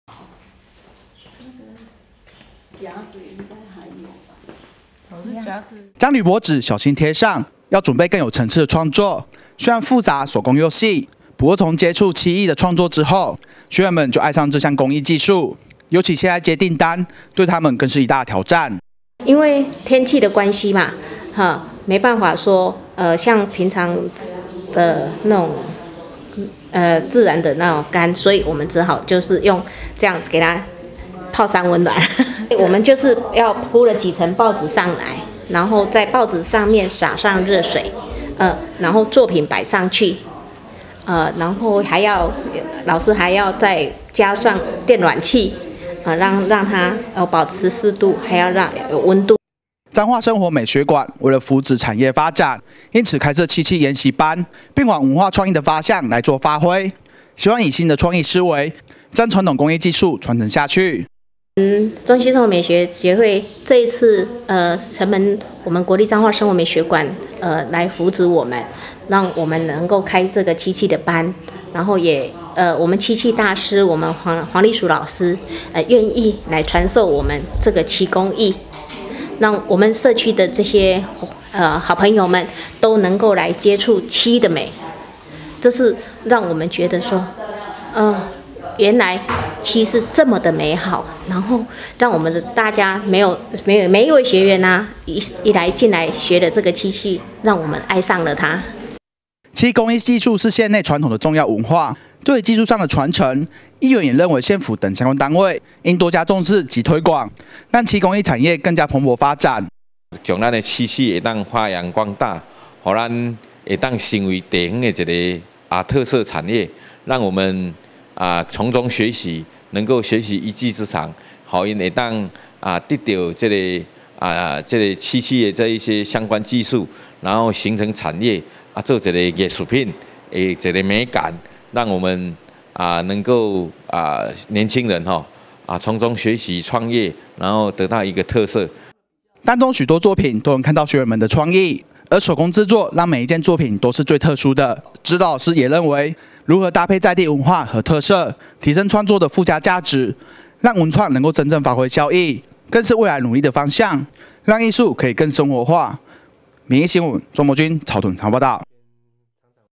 1050225 民議新聞 漆器研習班 推廣縣內傳統工藝技術文化(議員 林永鴻)
為了推廣傳統工藝文化，國立彰化生活美學館，日前開設漆器研習班，邀請漆器老師黃麗淑進行傳授，成果相當好，未來也希望結合文創方向發展，讓藝術更生活化，議員也期盼未來相關單位，應更加重視及推廣，讓傳統工藝技術，能在年輕一輩中流傳下去。